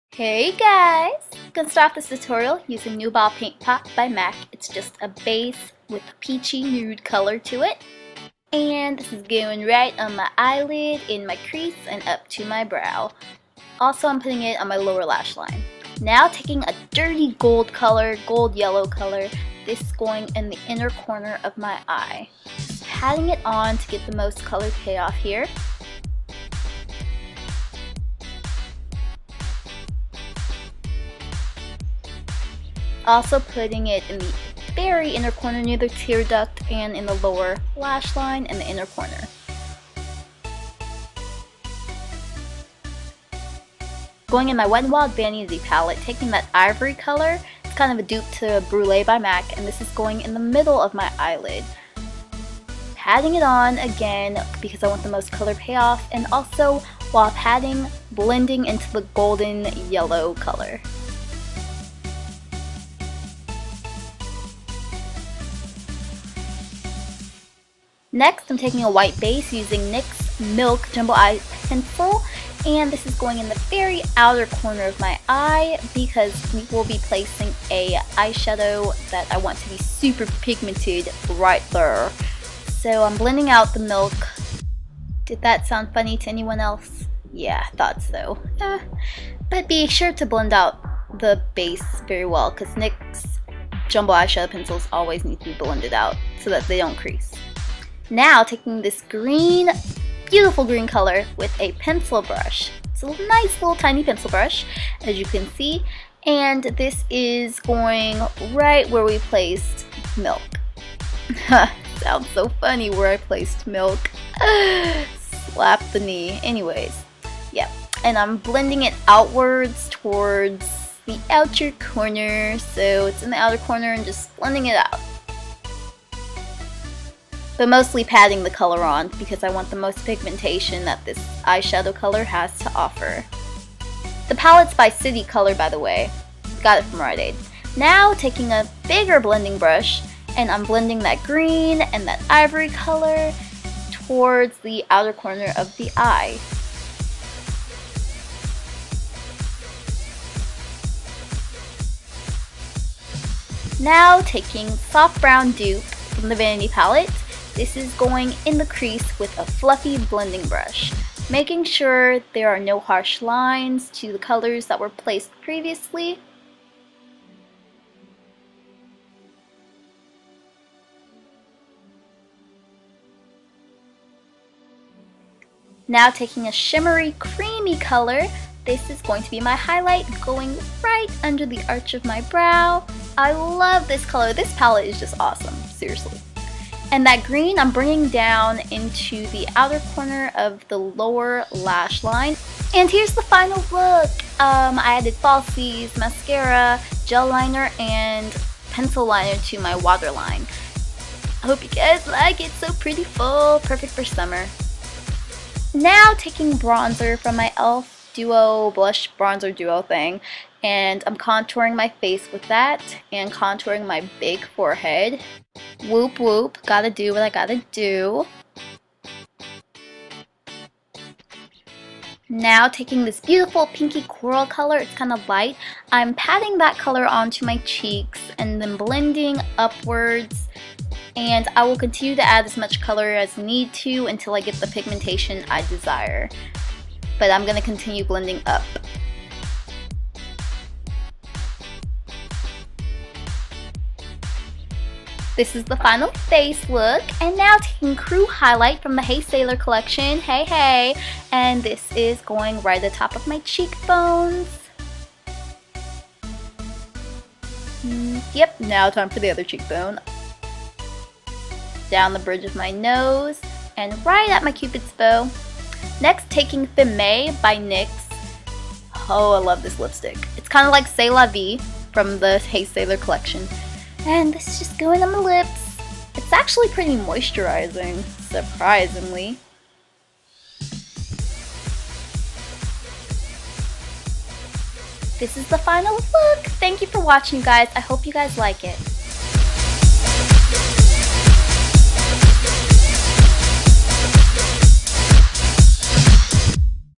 MAC Hey Sailor Collection Inspired Makeup Tutorial!
Hey loves!!! This tutorial was inspired by the Mac "Hey Sailor!" Collection and the products that were released in it. I used mostly budget-friendly dupes of the Mac Hey Sailor products, but I did use the Crew highlight! I hope you guys enjoy this tutorial! :D

Here's a list of Hey Sailor products you can use for this look instead of the dupes I used:
- Barefoot eyeshadow OR Old Gold pigment
- Feeling Fresh eyeshadow
- Launch Away! blush
- Sail La Vie lipstick

Pictures & full list of all products used will be on my blog soon!